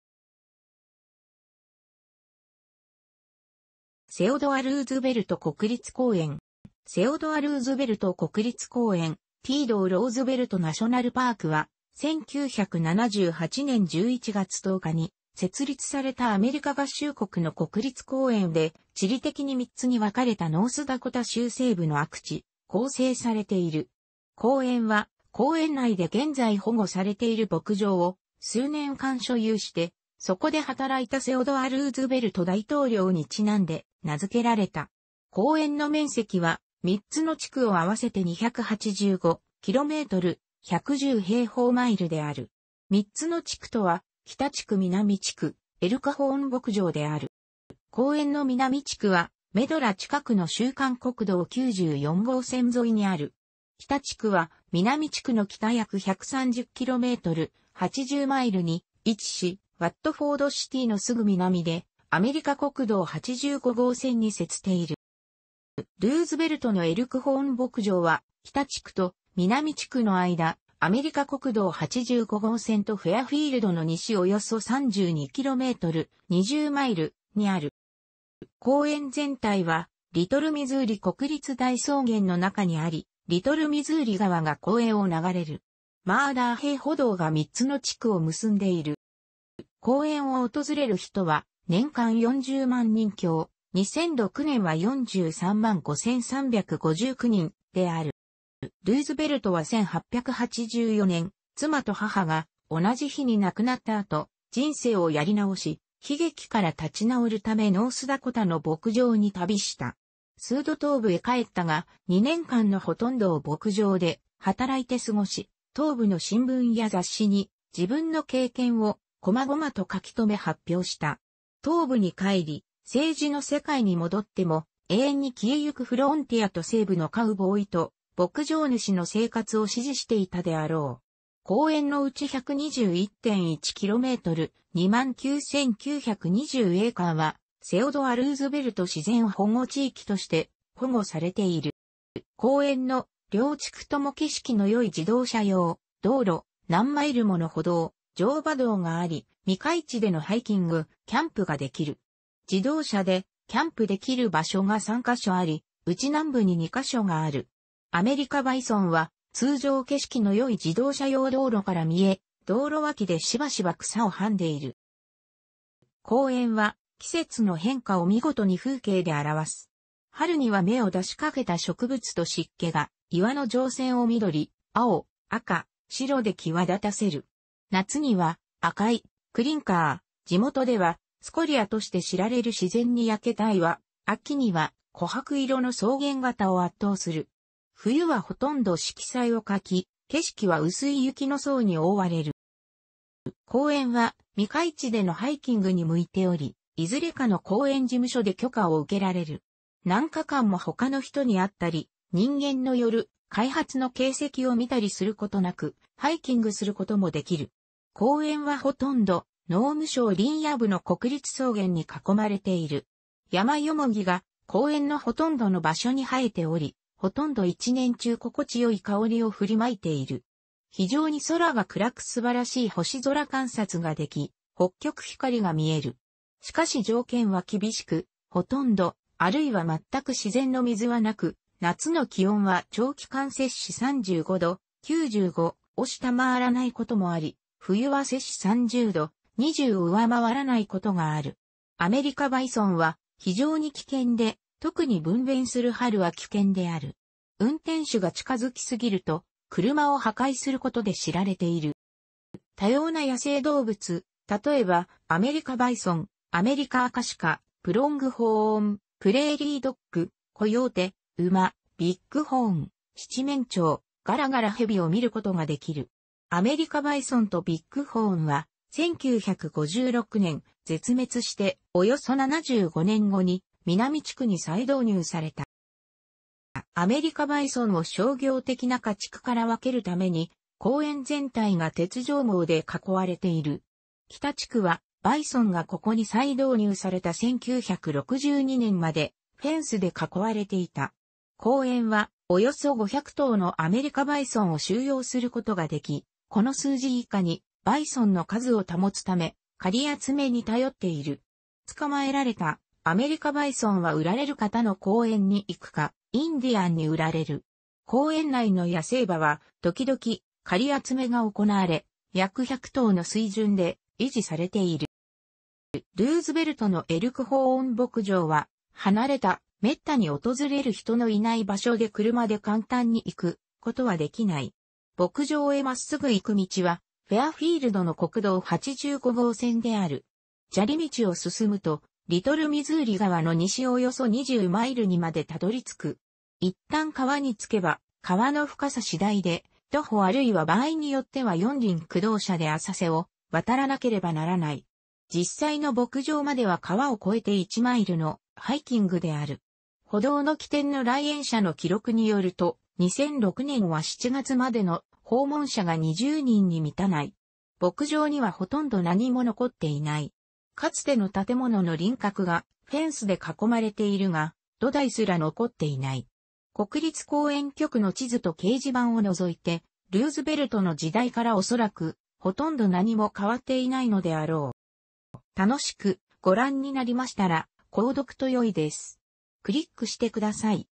セオドア・ルーズベルト国立公園
セオドア・ルーズベルト

セオドア・ルーズベルト国立公園（Theodore Roosevelt National Park）は、1978年11月10日に設立されたアメリカ合衆国の国立公園で、地理的に3つに分かれたノースダコタ州西部の悪地で構成されている。公園は、公園内で現在保護されている牧場を数年間所有してそこで働いたセオドア・ルーズベルト大統領にちなんで名付けられた。公園の面積は、3つの地区を合わせて285 km（110平方マイル）である。3つの地区とは、北地区 (North Unit) 、南地区 (South Unit) 、エルクホーン牧場 (Elkhorn Ranch) である。

公園の南地区は、メドラ近くの州間国道94号線沿いにある。北地区は南地区の北約130km（80マイル）に位置し、ワットフォード・シティー (Watford City) のすぐ南でアメリカ国道85号線に接している。

ルーズベルトのエルクホーン牧場は北地区と南地区の間、アメリカ国道85号線とフェアフィールド (Fairfield)の西およそ32km（20マイル）にある。

公園全体は、リトル・ミズーリ国立大草原 (Little Missouri National Grassland) の中にあり、リトルミズーリ川が公園を流れる。マー・ダー・ヘイ歩道 (Maah Daah Hey Trail) が3つの地区を結んでいる。

公園を訪れる人は、年間400,000人強（2006年は435,359人）である。

ルーズベルトは1884年、妻と母が（同じ日に）亡くなった後、人生をやり直し、悲劇から立ち直るためノースダコタの牧場に旅した。数度東部へ帰ったが、2年間のほとんどを牧場で働いて過ごし、東部の新聞や雑誌に自分の経験をこまごまと書き留め発表した。東部に帰り、政治の世界に戻っても、永遠に消えゆくフロンティアと西部のカウボーイと牧場主の生活を支持していたであろう。
公園のうち121.1 km（29,920 エーカー）はセオドア・ルーズベルト自然保護地域 (Theodore Roosevelt Wilderness) として、保護されている。

公園の両地区とも景色の良い自動車用道路、何マイルもの歩道、乗馬道があり、未開地でのハイキング、キャンプができる。自動車でキャンプできる場所が3箇所あり、うち南部に2箇所がある。アメリカバイソンは、通常景色の良い自動車用道路から見え、道路脇でしばしば草を食んでいる。

公園は、季節の変化を見事に風景で表す。春には芽を出しかけた植物と湿気が、岩の条線を緑、青、赤、白で際立たせる。夏には、赤い「クリンカー」 ("clinker") （地元ではスコリア ("scoria") として知られる自然に焼けた岩）、秋には琥珀色の草原が他を圧倒する。冬はほとんど色彩を欠き、景色は薄い雪の層に覆われる。

公園は、未開地でのハイキングに向いており、いずれかの公園事務所で許可を受けられる。何日間も他の人に会ったり、人間のよる開発の形跡を見たりすることなく、ハイキングすることもできる。公園はほとんど、農務省林野部 (United States Forest Service) の国立草原 (National Grassland) に囲まれている。ヤマヨモギが公園のほとんどの場所に生えており、ほとんど一年中心地よい香りを振りまいている。非常に空が暗く素晴らしい星空観察ができ、北極光が見える。しかし条件は厳しく、ほとんど、あるいはまったく自然の水はなく、夏の気温は長期間35℃（95）を下回らないこともあり、冬は −30℃（−20）を上回らないことがある。アメリカバイソンは非常に危険で、特に分娩する春は危険である。運転手が近づきすぎると、車を破壊することで知られている。

多様な野生動物、例えばアメリカバイソン、アメリカアカシカ、プロングホーン、プレーリードッグ、コヨーテ、ウマ、ビッグホーン、シチメンチョウ、ガラガラヘビを見ることができる。アメリカバイソンとビッグホーンは、1956年、絶滅しておよそ75年後に南地区に再導入された。

アメリカバイソンを商業的な家畜から分けるために、公園全体が鉄条網で囲われている。北地区は、バイソンがここに再導入された1962年までフェンスで囲われていた。公園は、およそ500頭のアメリカバイソンを収容することができ、この数字以下にバイソンの数を保つため、駆り集め...